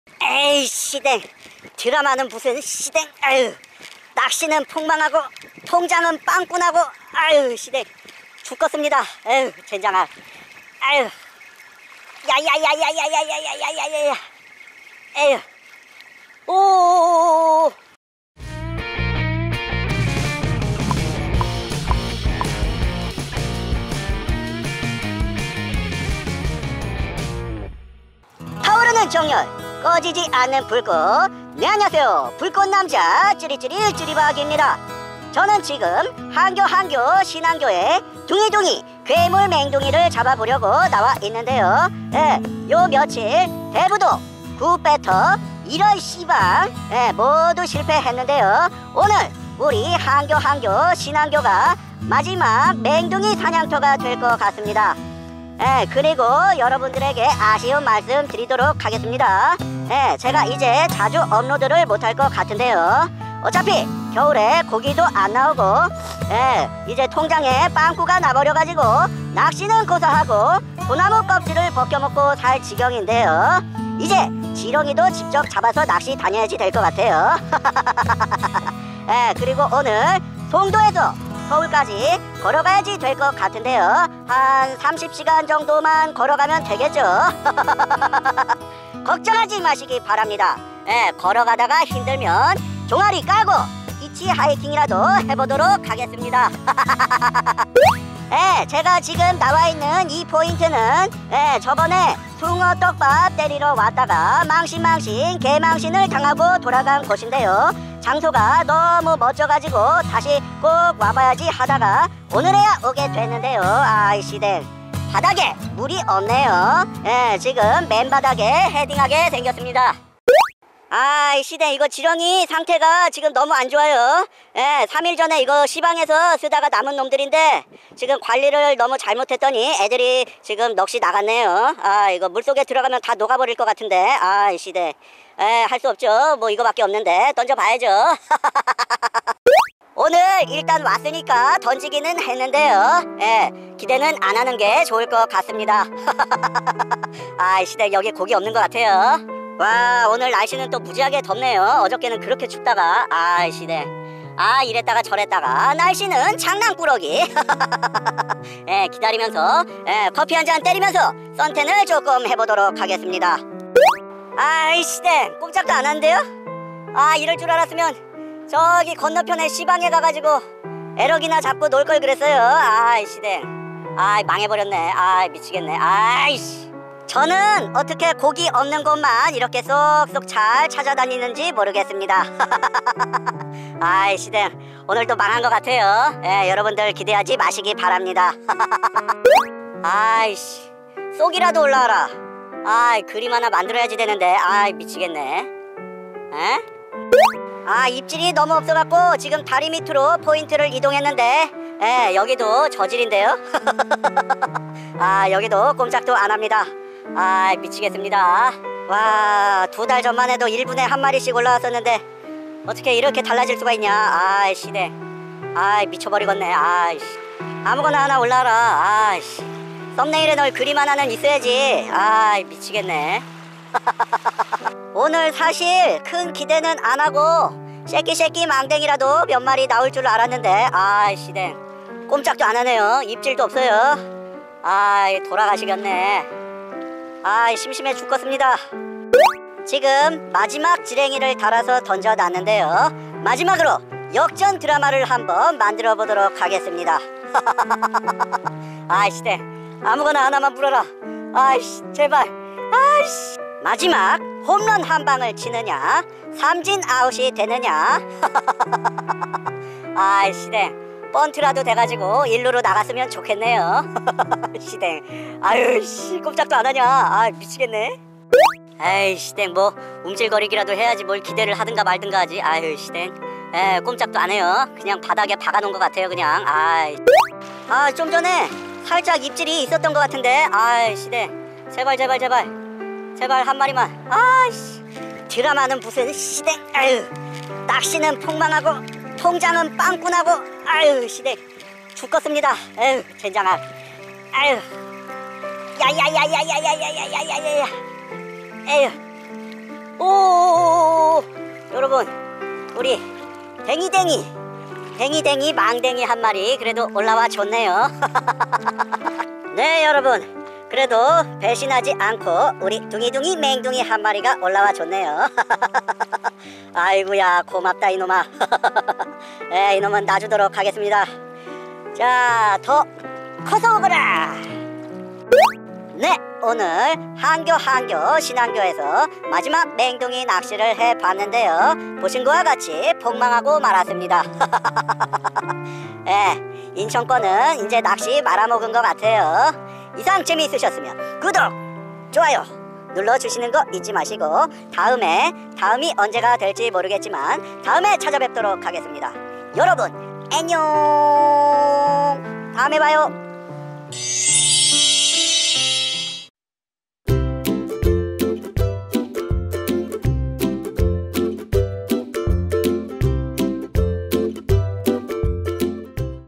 [ 쯔리박(つり朴) ] 여기도 안나오면 인천권 망뎅이낚시는 폭망각! (신항교낚시,인천낚시,송도낚시)
망뎅이밭인 신항교도 망뎅이가 안나오면 인천권은 전부 망했다고 봅니다~ 씨뎅~ ㅜㅜ